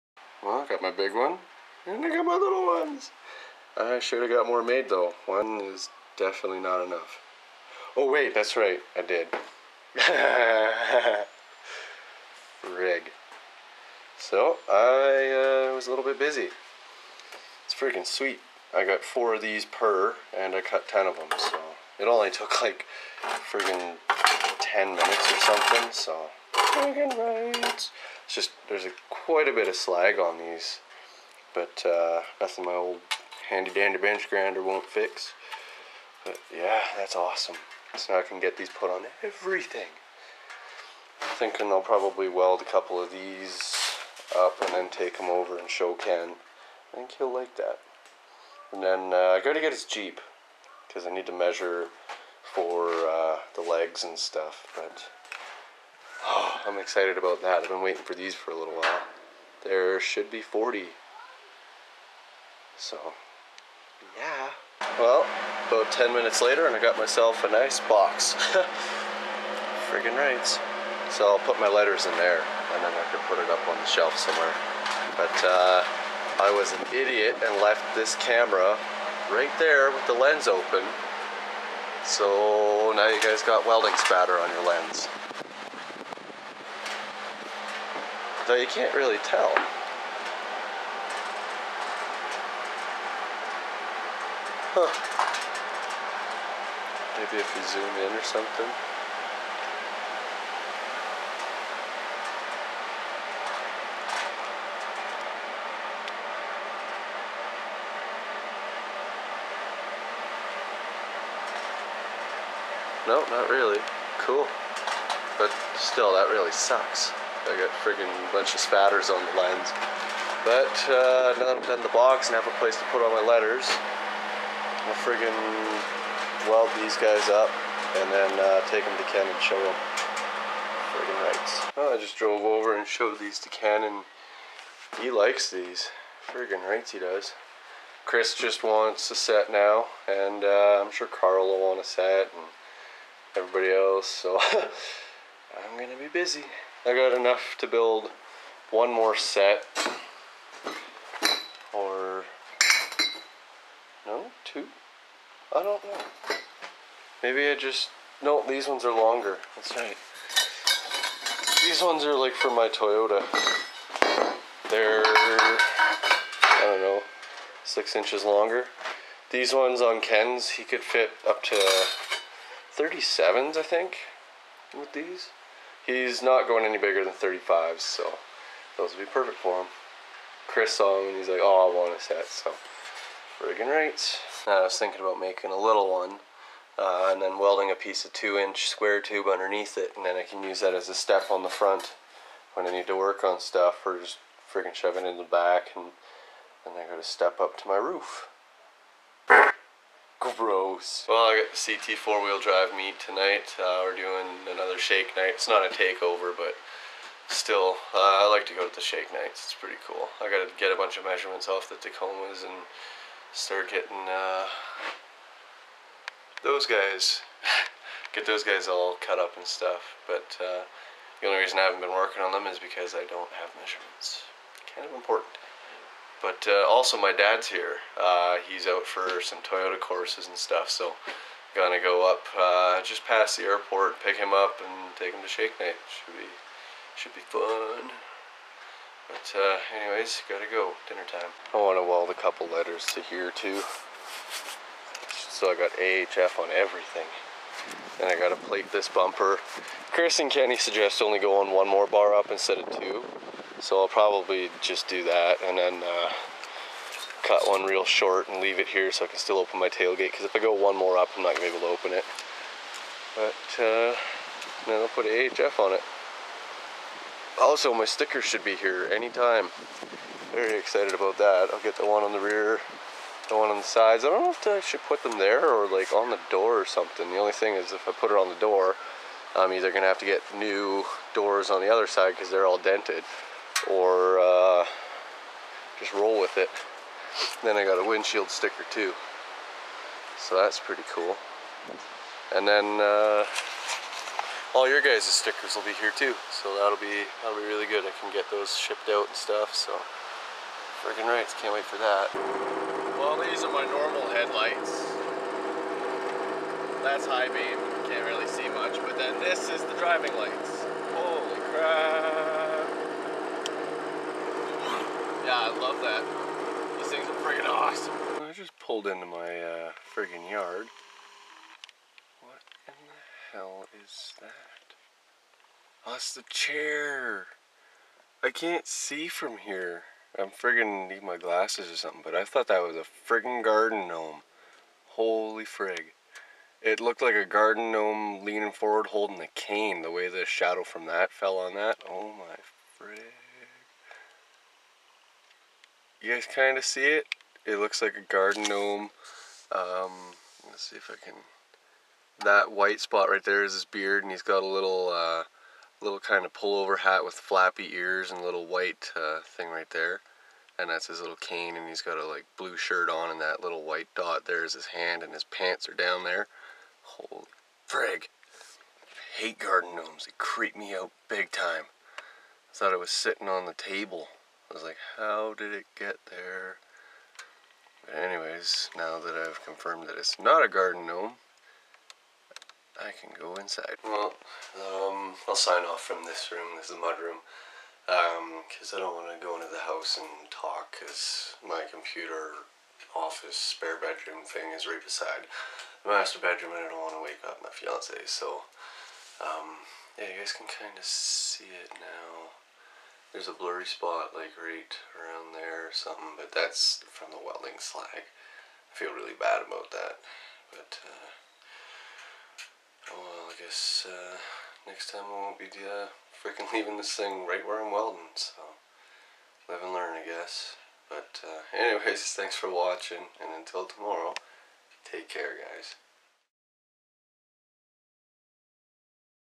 AHF Letters, Step Bars & Creepy Chair Gnomes... (06/06/12)
Well guys, i got some more AHF letters cut out, only a couple though ;) haha

then i got some welded to Ken's step bars. Kris ordered a set, so ill be making at least 1 more set of these.

Nanton, AB
T0L1R0